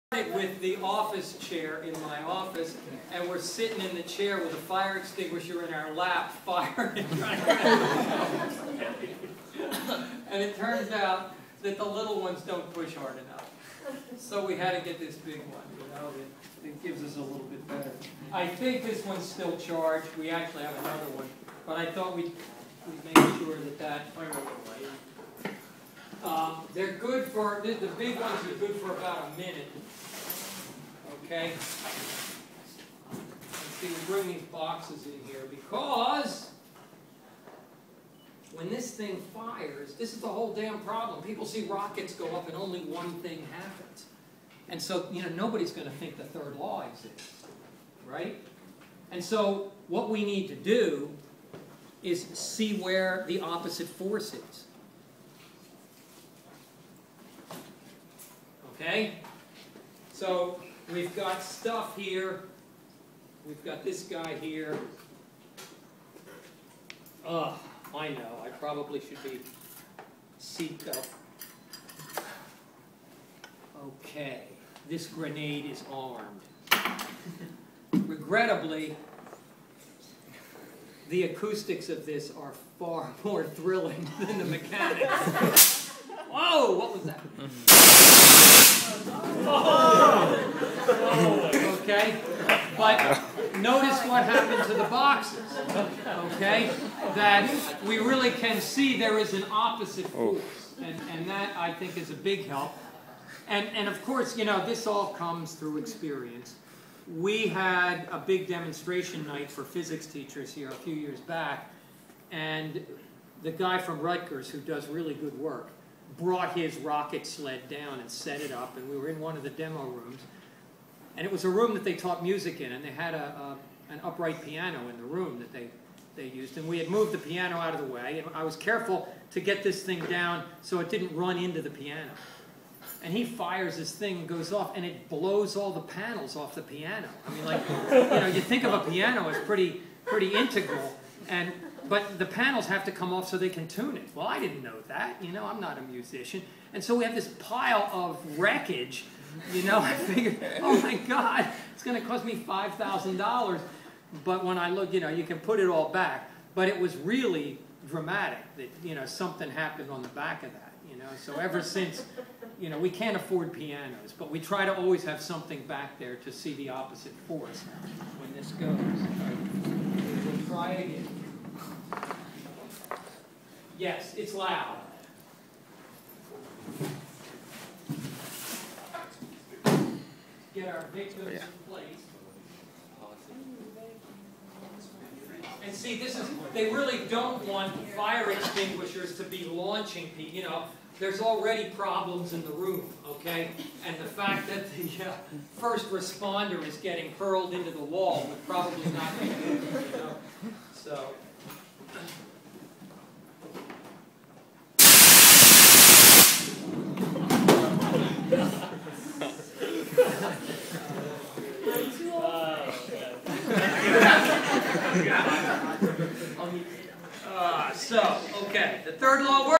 Physics Teacher Rides a Fire Extinguisher
Yup. It's what we do in physics class everyday. More or less...